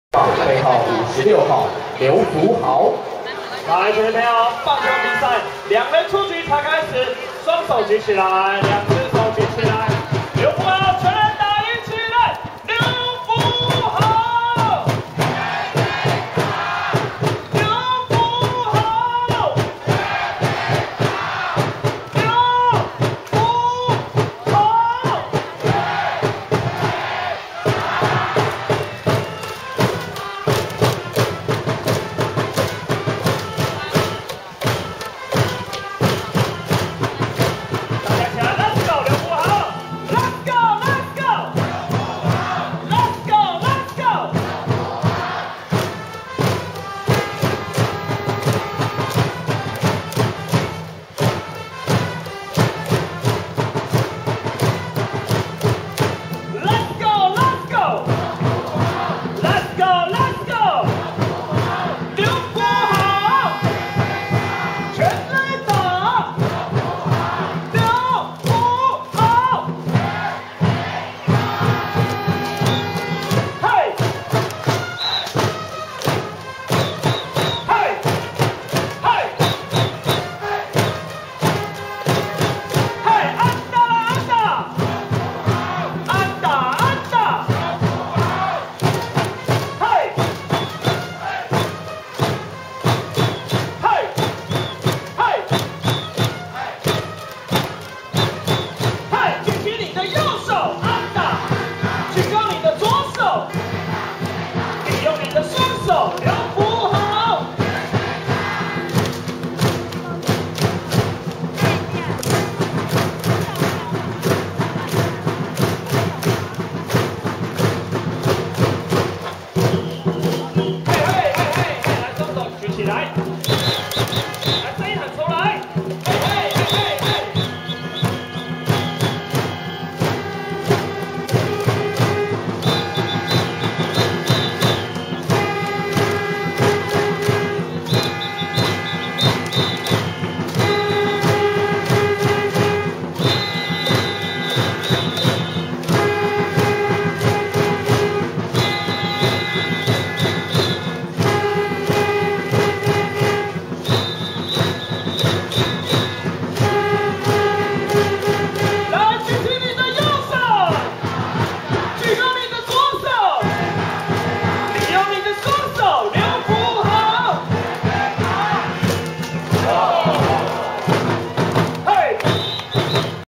140606 - 台南 - 統一獅 - 7下 - 劉芙豪 - 應援 (4)
第二棒 RF 劉芙豪：四壞球保送，攻佔一壘。